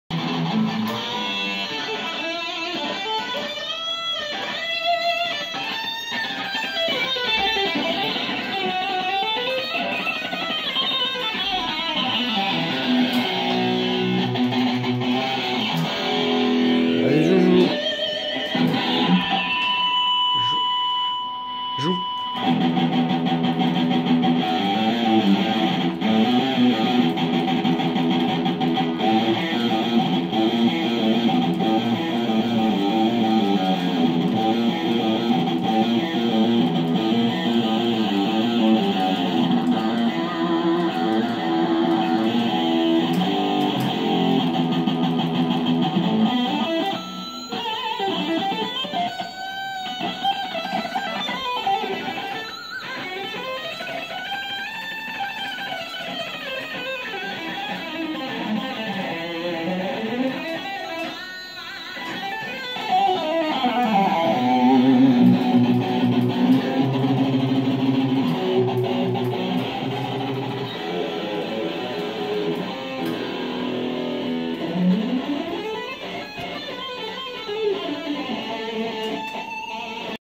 Low Latency WebAudio with Chrome 67 stable + Windows 10 + Guitar Amp Sim Designer
This video shows that windows 10 greatly improve its audio driver. Here you can see Chrome 67 stable version on Windows 10, running the WebAudio Guitar amp sim designer tool we created for the WASABI research project. A fast guitar player can enjoy playing, latency is barely noticeable. A little bit higher that what we get on Mac OS. Presonus sound card